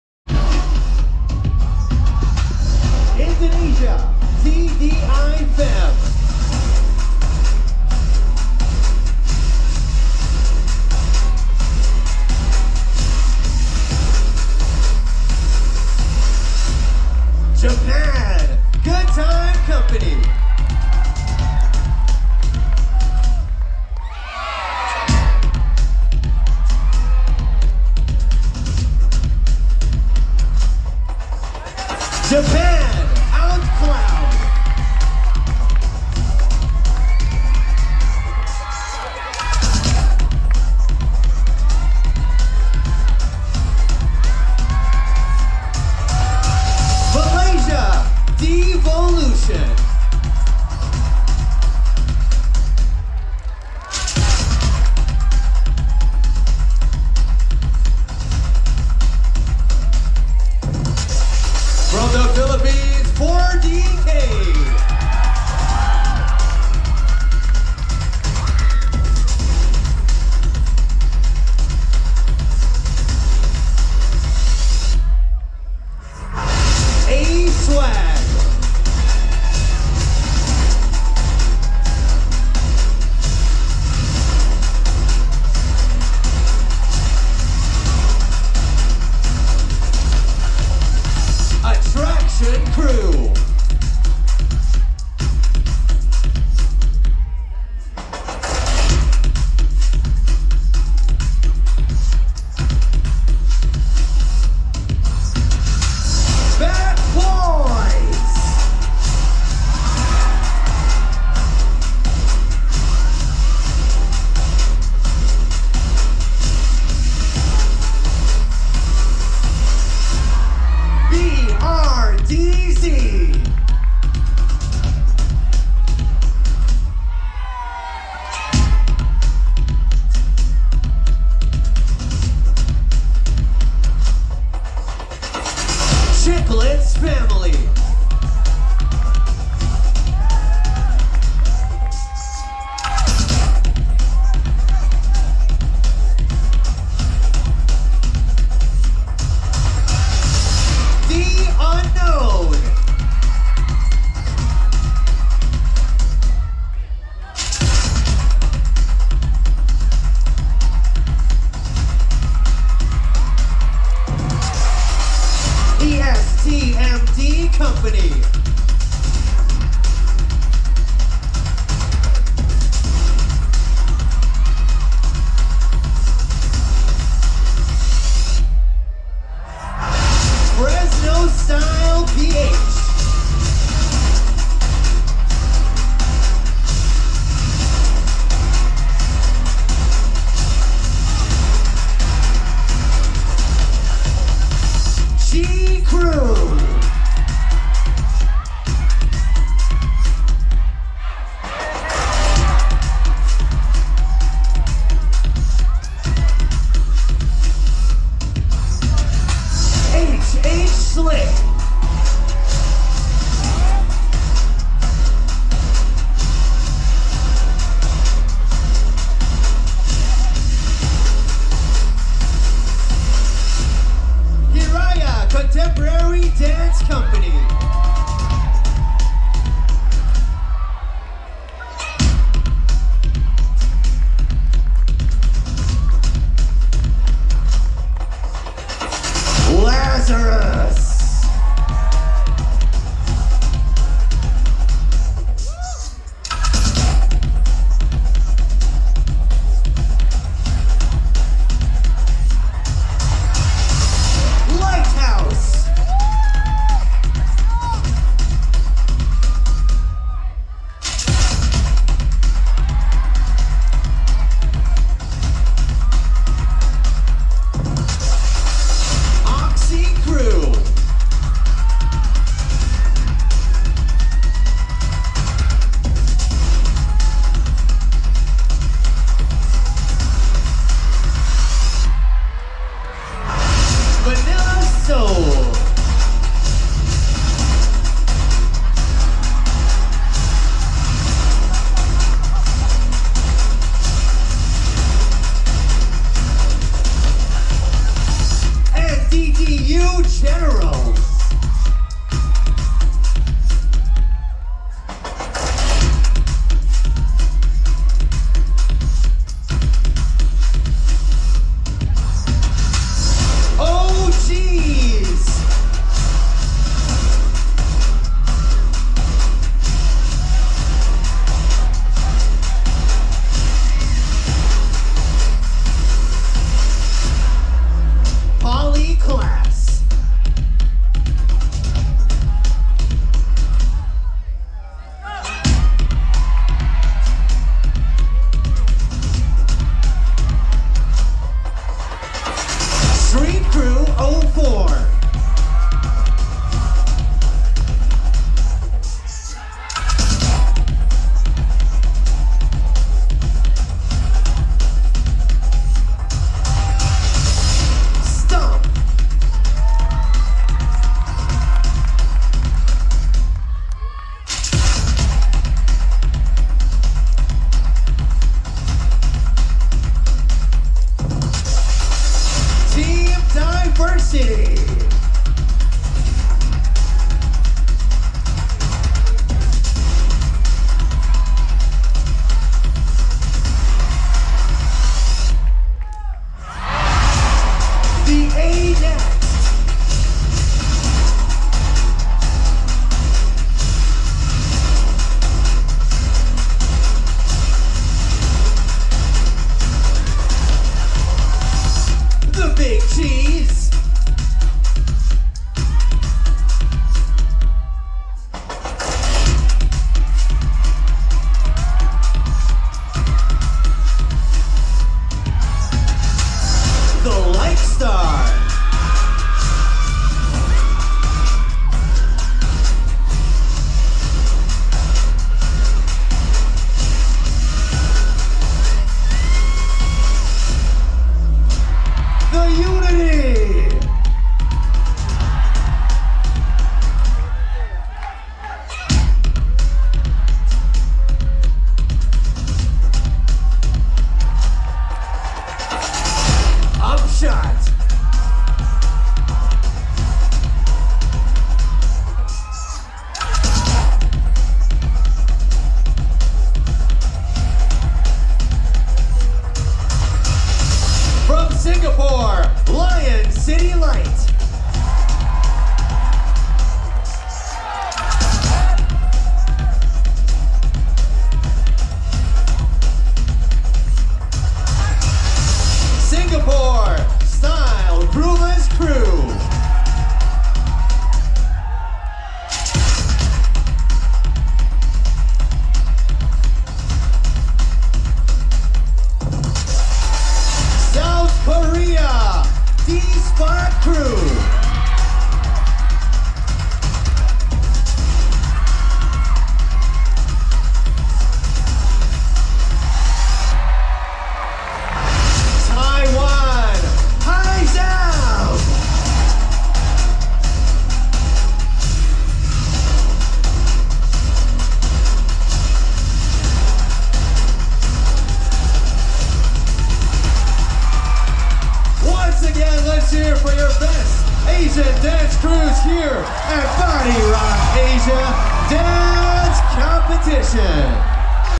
BODY ROCK ASIA OPENING PRODUCTION - (BODY ROCK ASIA DANCE COMPETITION 2023) 02/25/23
BODY ROCK ASIA DANCE COMPETITION 2023 @ The Theatre Solaire Aseana Avenue Entertainment City Paranaque City

CHAMPION- CHIKLETZ FAMILY
1st RUNNER UP-  STREET CREW 04
2nd RUNNER UP- OXYCREW